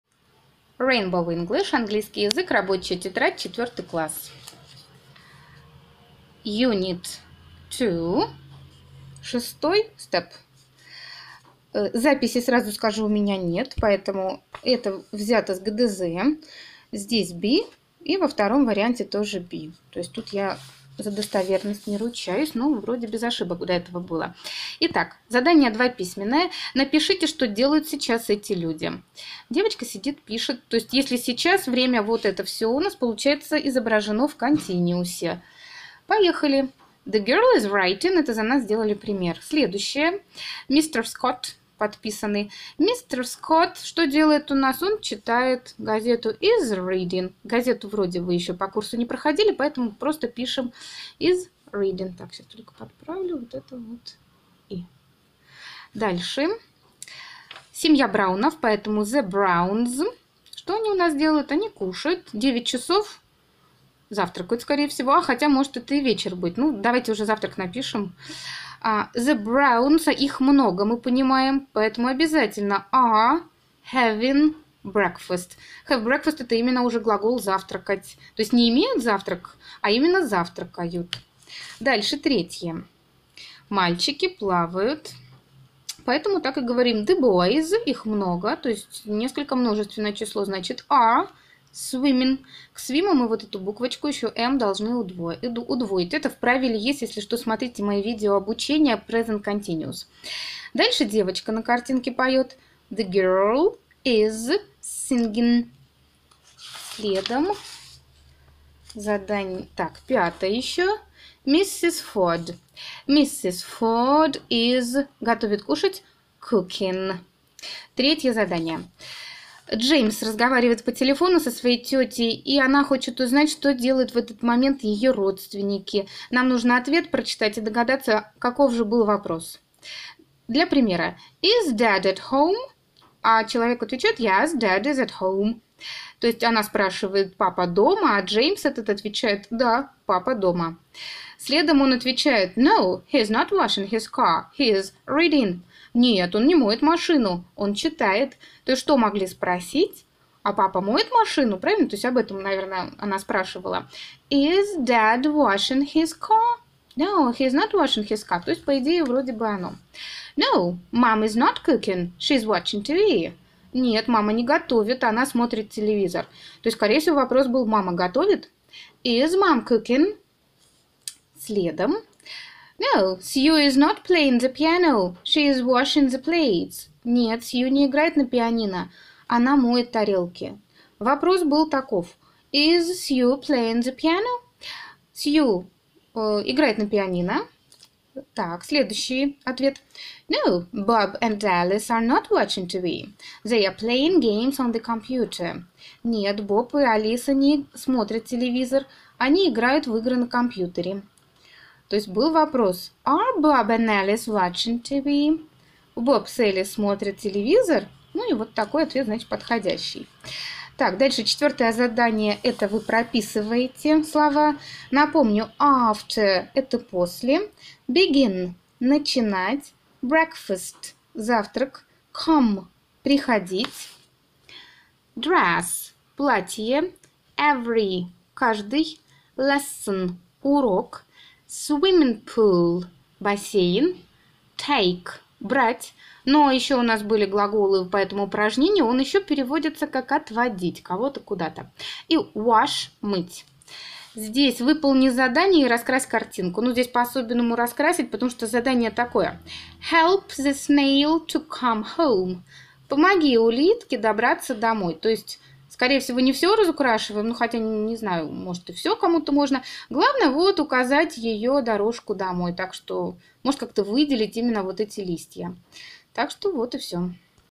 Unit 2, Step 6 / ГДЗ. Rainbow English. 4 класс. Рабочая тетрадь
ГДЗ. Ответы к рабочей тетради Rainbow English. Английский язык. 4 класс/ О.В.Афанасьева, И.В.Михеева. - М., 2017